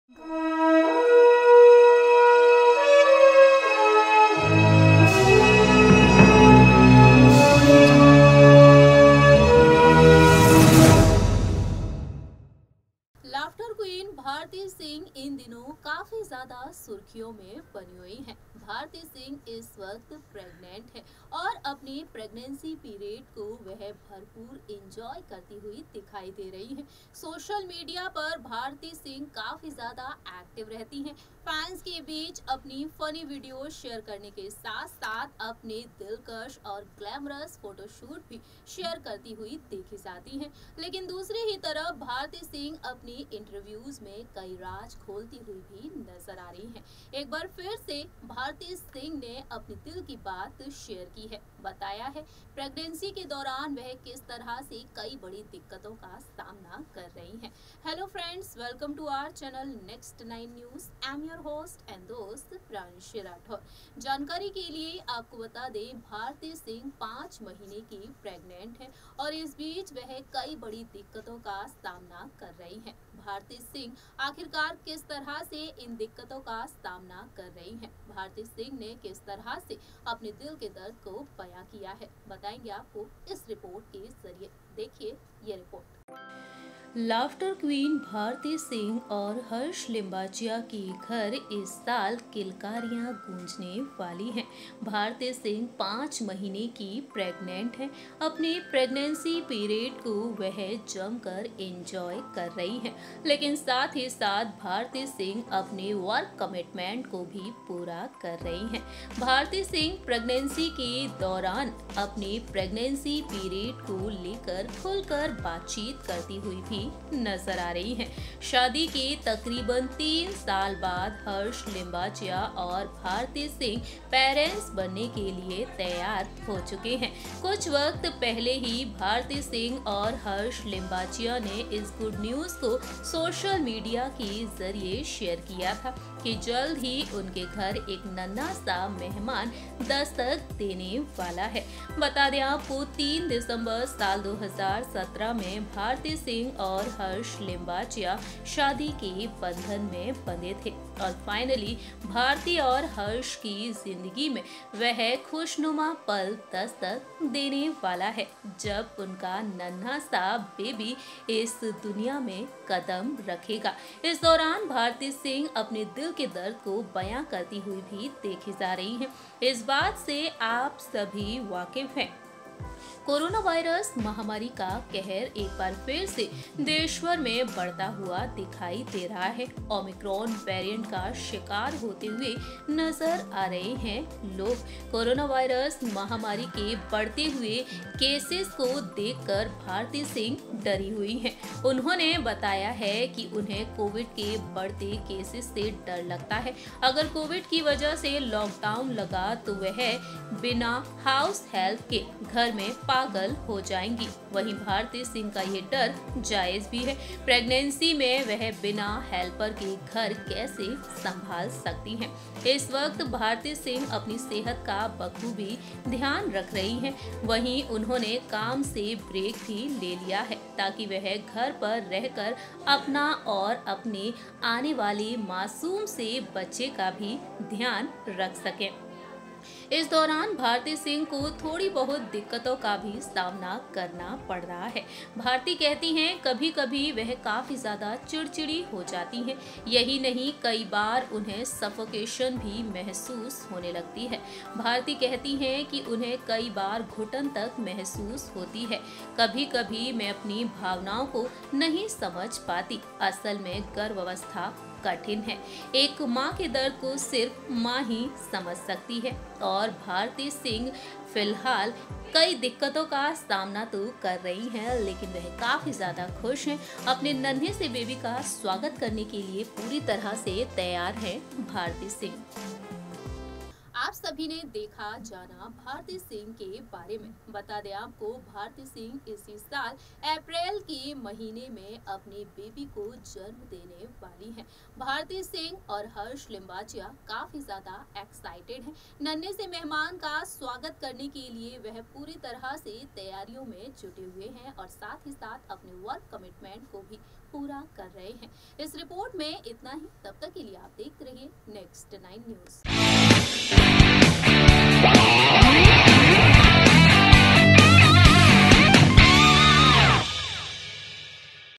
Pregnancy के दौरान भारती सिंह की हुई इतनी बुरी हालत, खुद बयां किया अपना दर्द| Bharti Singh
कॉमेडियन भारती सिंह इन दिनों प्रेग्नेंसी पीरियड को जमकर इंजॉय कर रही हैं. प्रेग्नेंसी के दौरान भारती सिंह कई बड़ी दिक्कतों का सामना कर रही हैं. हाल ही में भारती ने खुद इस बात का खुलासा किया है